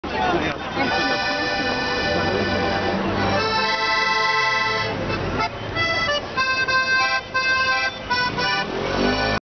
A little boy's music in Rome
Many a times small experiences touch you and tug at your heartstrings, this is one of those.